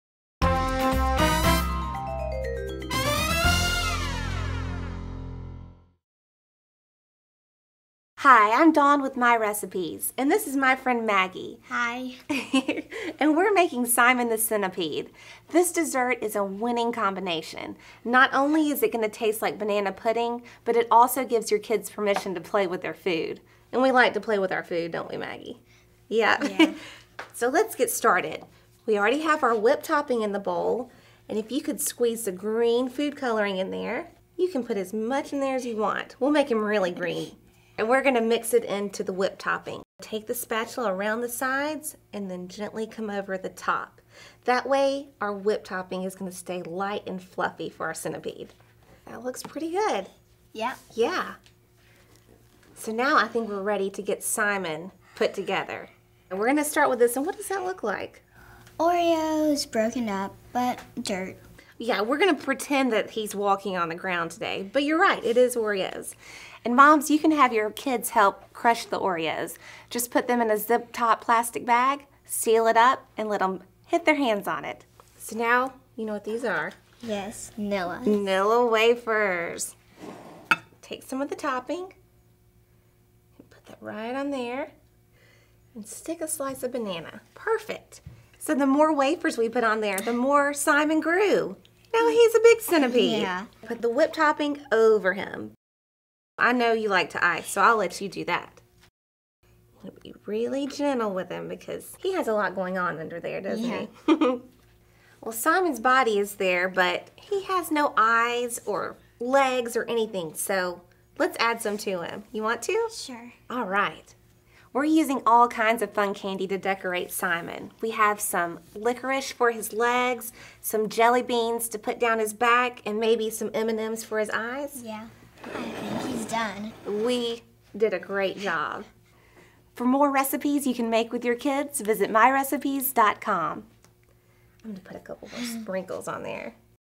Simon the Centipede Recipe - Kids Love It!
Let your kids help make and decorate this little creature that tastes like banana pudding.

"It was fun mixing up the green stuff. I'd love to make this again, and when I do, I might use a different color for the icing." --Taylor, Age 6

Prep: 25 min.; Other: 8 hrs.

Yield: Makes 6 servings
Ingredients

    * 1  (8-ounce) container frozen whipped topping, thawed
    * 1/2  to 1 teaspoon green liquid food coloring
    * 36  vanilla wafers
    * 1  banana, peeled and cut into 36 slices
    * Decorations*

Preparation

1 Place whipped topping in a medium bowl; gently fold in food coloring.

2 Place 1 teaspoon whipped topping on the flat side of 1 vanilla wafer; top with a banana slice. Do the same thing all over again with the whipped topping, vanilla wafers, and banana slices. You'll have 36 stacks. Put 6 stacks together in a row to make a log. Curve log slightly, if you'd like. Do the same thing all over again with the remaining stacks. Spread remaining whipped topping on each log. Cover and chill in the refrigerator at least 8 hours.

3 Attach chocolate pieces to logs to resemble eyes and spots. Cut fruit roll into small pieces, and attach to logs to resemble mouths. To make the antennae and legs: Snap spaghetti strands in half. Separate candy ropes into strands. Cut strands into 12 (7-inch) segments. Tie a loose knot into 1 candy strand close to the end. Insert the tip of 1 strand of spaghetti into the knot, and carefully tighten. Starting at the knot, wind the candy strand around the spaghetti pressing gently as you go so the candy will stick to the spaghetti. Break the spaghetti off about an inch below the strand. Do the same thing all over again with the remaining 11 (7-inch) segments of candy and spaghetti. Insert into the logs just above the eyes to resemble antennae. Separate more candy ropes into thin strands. Cut strands into 6 dozen (1-inch) segments, and stick them into sides of logs to resemble legs.

4 Serve on a bed of crumbled cream-filled sandwich cookies, if you'd like.

Note: This cute dessert tastes a lot like banana pudding. The crisp vanilla wafers soften as it chills overnight, so it's perfect to eat with a spoon.

*Decorations: miniature candy-coated chocolate pieces (we used M&M's minis), 1 cherry-flavored chewy fruit roll (we used Fruit Rollups), 6 uncooked spaghetti strands, pull-and-peel red candy ropes (we used Twizzlers), crumbled cream-filled chocolate sandwich cookies (optional)
Nutritional Information

Calories:
    282 (41% from fat)
Fat:
    13g (sat 6.6g,mono 0.0g,poly 0.0g)
Protein:
    2.6g
Carbohydrate:
    36.3g
Fiber:
    0.5g
Cholesterol:
    6mg
Iron:
    1.4mg
Sodium:
    156mg
Calcium:
    1mg

Oxmoor House, MAY 2007